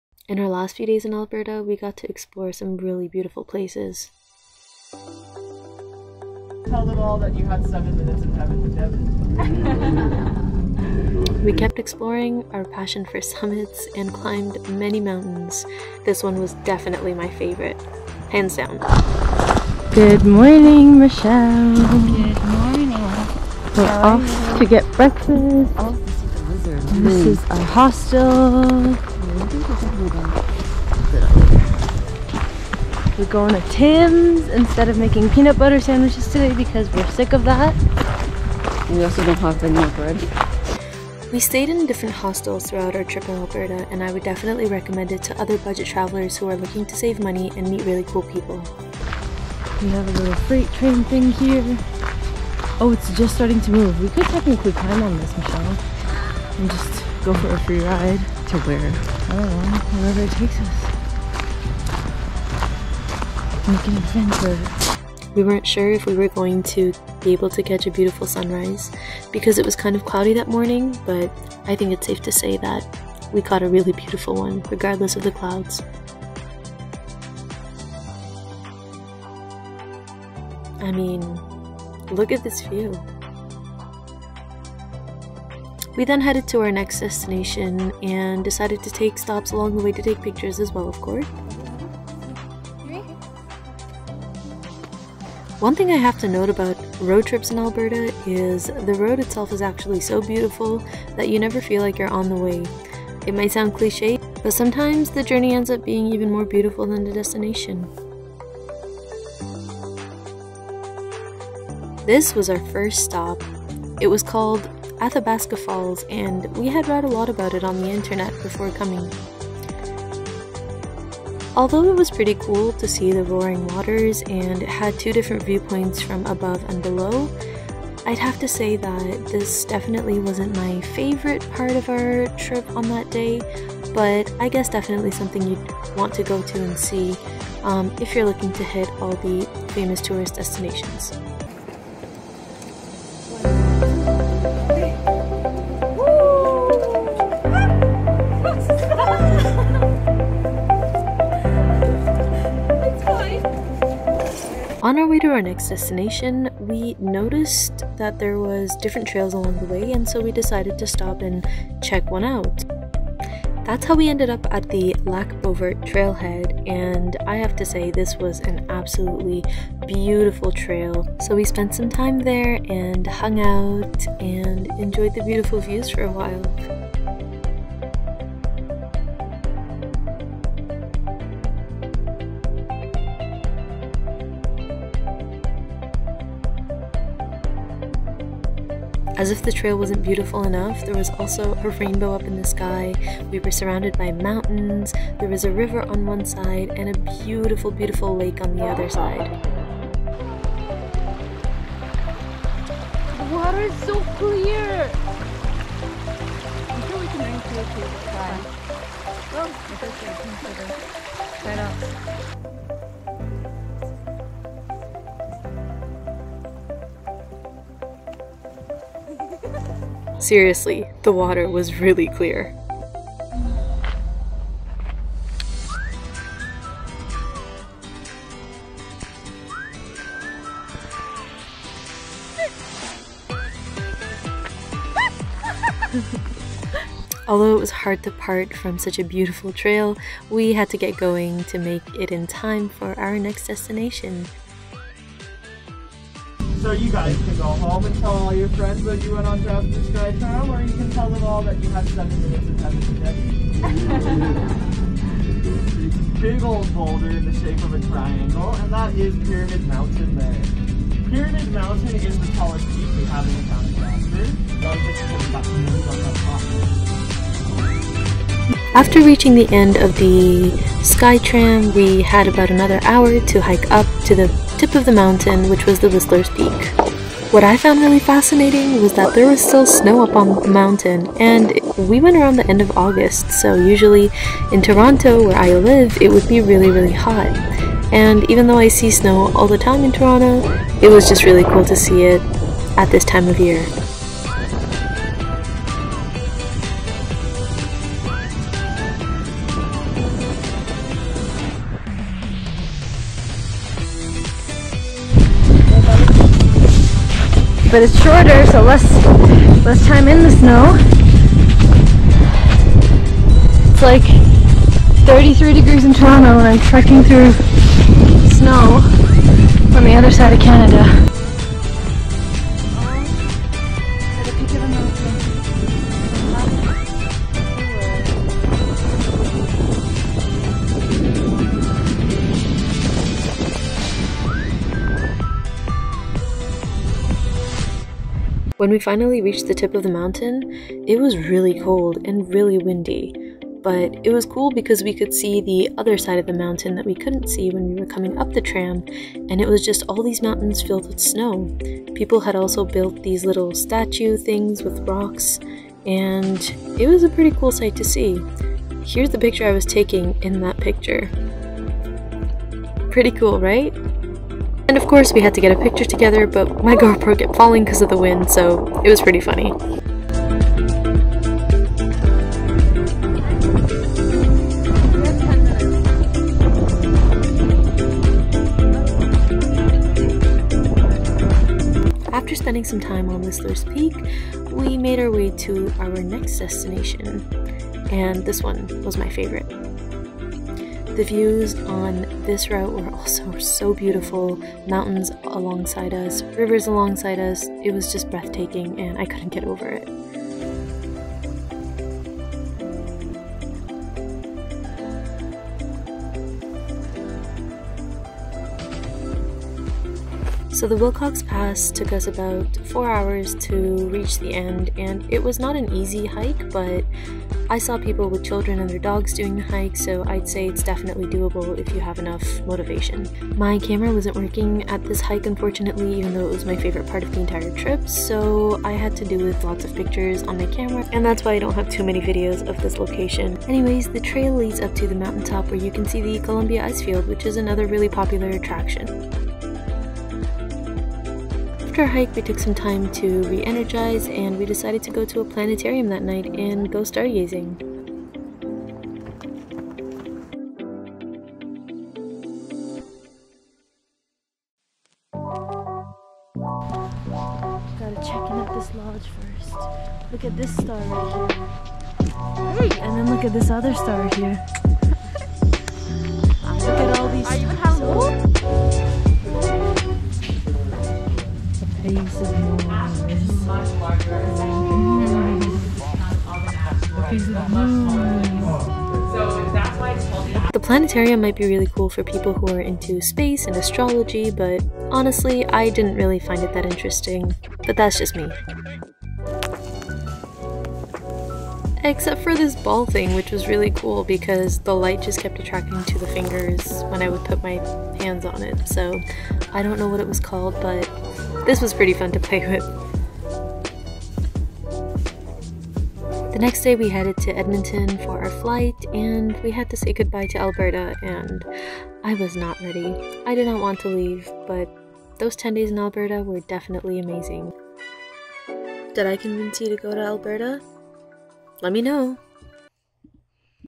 5 Places you HAVE to see when visiting Jasper, Alberta
From popular attractions to hidden gems, here's an honest guide to Jasper's most amazing places. Have you been to Jasper?